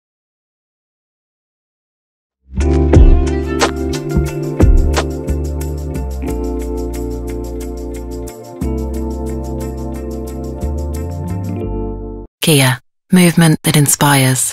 Kia360 | The Inspiration Hub | Live Music Performance
Ready for yet another weekend full of inspiration and melodies?
Show up to a rare and wild live music event with Hanita Bhambri at The Inspiration Hub – Kia360 on 17th December 2022, from 5 pm onwards.

While you are at it have a look at the most rare and wild machine – Sonet X-Line.